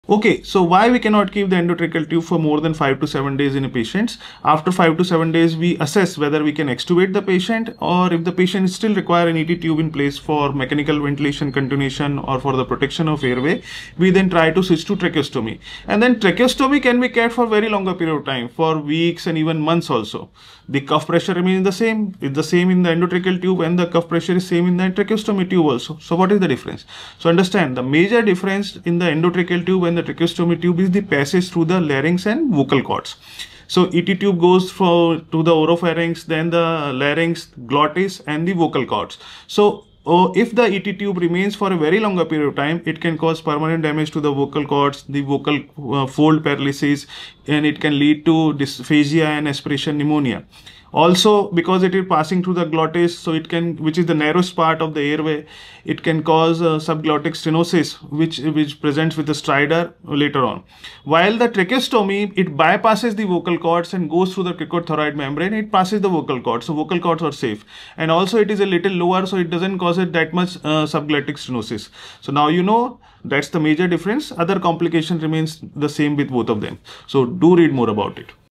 icushort 357: Why ET tube can't be kept more than 5-7 days while TT tube can be kept for longer
Why endotracheal tube can't be kept more than 5-7 days while tracheostomy tube can be kept for weeks and months